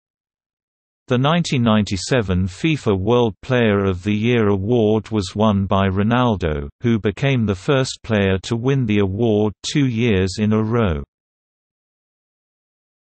1997 FIFA World Player of the Year | Wikipedia audio article
- Socrates

SUMMARY
The 1997 FIFA World Player of the Year award was won by Ronaldo, who became the first player to win the award two years in a row.